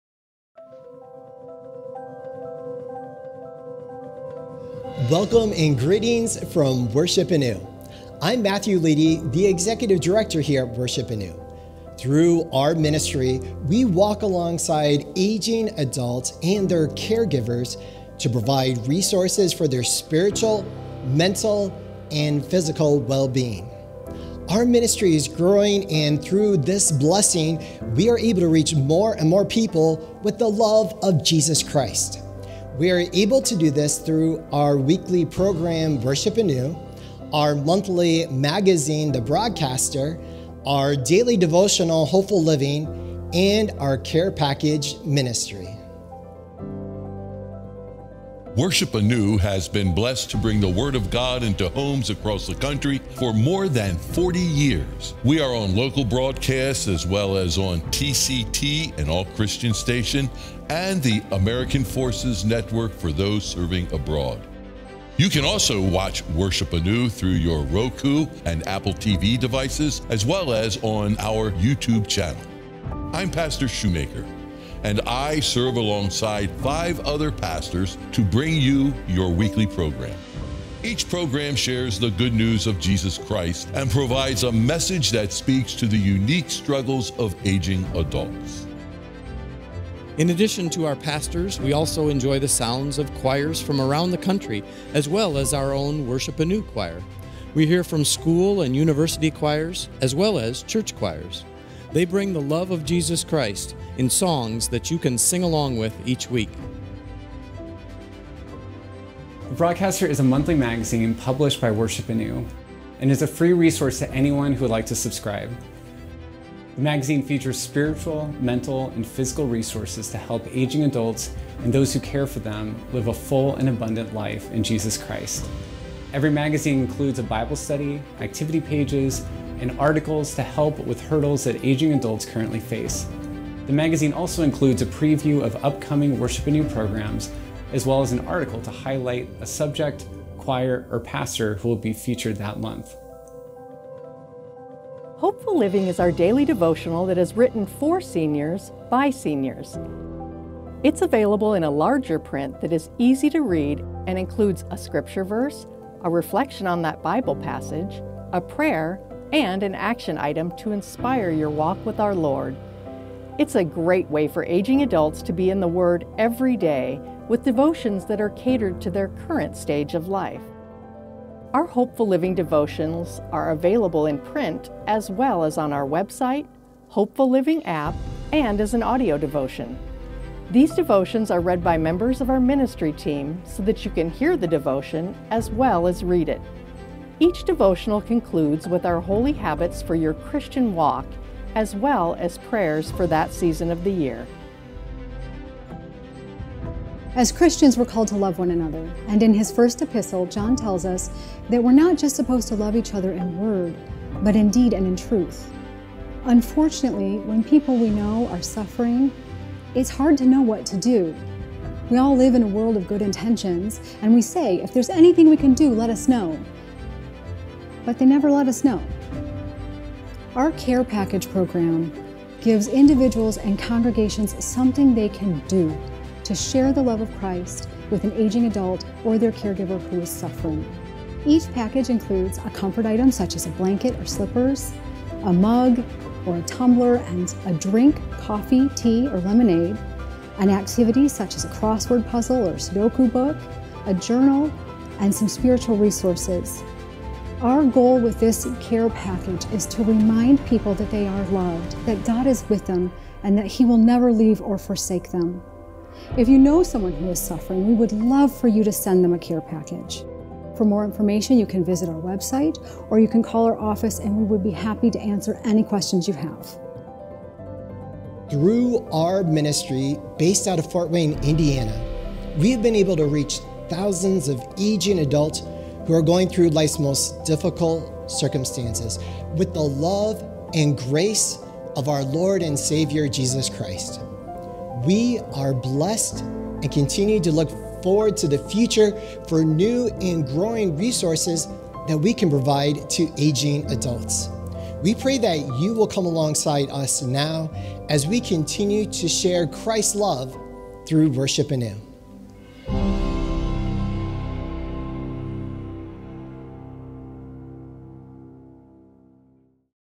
Learn more about Worship Anew
Worship Anew engages and equips aging adults with resources to live a full and abundant life through Jesus Christ. We are able to do that through our weekly program, care package ministry, Broadcaster magazine, and Hope-Full Living devotional. Hear from our team more about these resources.